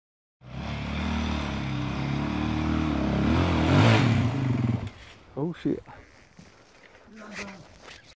Gunnison, CO - 5
Motorcycle single track north of Gunnison, Colorado. Very steep with lots of rocks and large exposed roots.